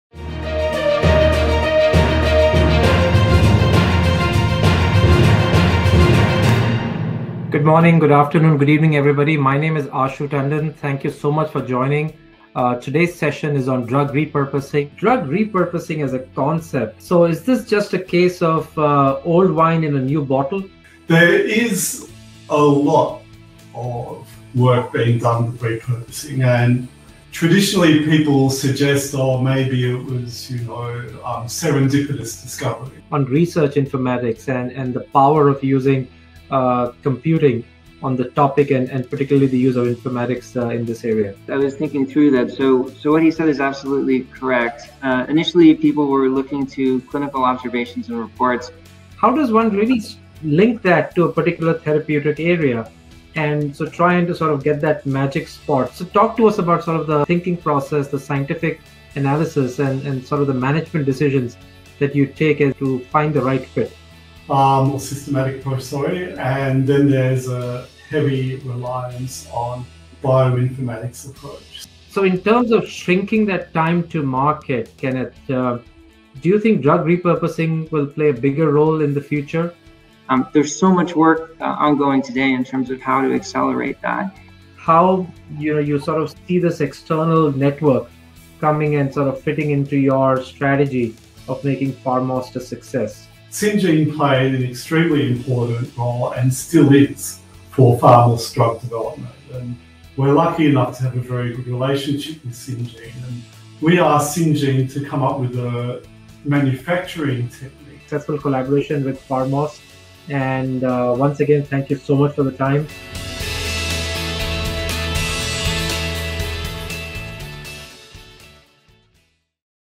Drug Repurposing — Perspective on an emerging pharmaceutical solution Promo
In this 60-minute interactive virtual exchange on Drug Repurposing — Perspective on an emerging pharmaceutical solution, we have Dr Richard Mollard, MBA, Ph.D., Chief Scientific Officer,  PharmAust Ltd in the hot seat, along with Kenneth Barr,  Ph.D., Senior Vice President - Discovery Services, Syngene, and Host, Ashu Tandon, CCO, Syngene.

During the session, they discussed.

•  Drug repurposing as an effective alternative approach to traditional Drug Discovery
•  Changing Landscape: Drug repurposing gaining considerable momentum
•  Steps in the Drug repurposing journey – moving to GMP scale
•  Australian Pharmaceutical Industry Landscape
•  Dynamics of working with an Indian CRO